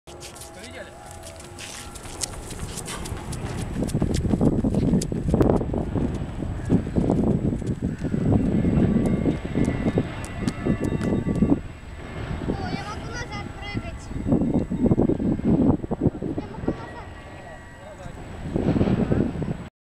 Грач Рики - полеты на прогулке
Уже очень неплохо на руку приземляется. Если я не сильно торможу, то он подруливает что приземлиться именно на руку.